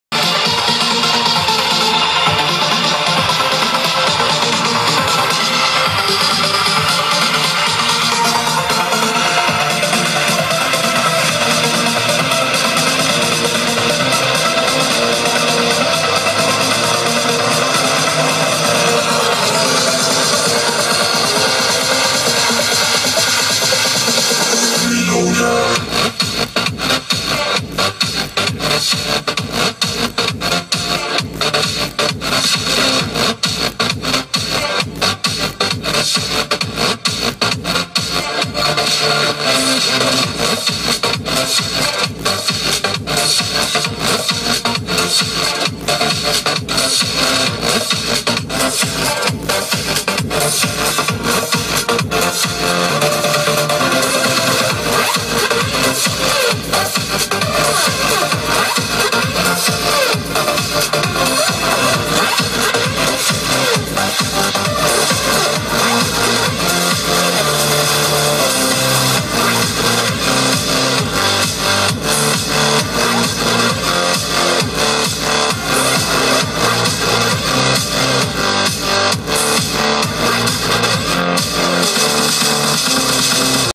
$160 subwoofer excursion
my $160 U-DIMENSION EL-S104 SUBWOOFER EXCURSION
DRIVEN BY RODEK RDS1500.1D 500WRMS(1Ohm) running at 4 Ohm @125 watt RMS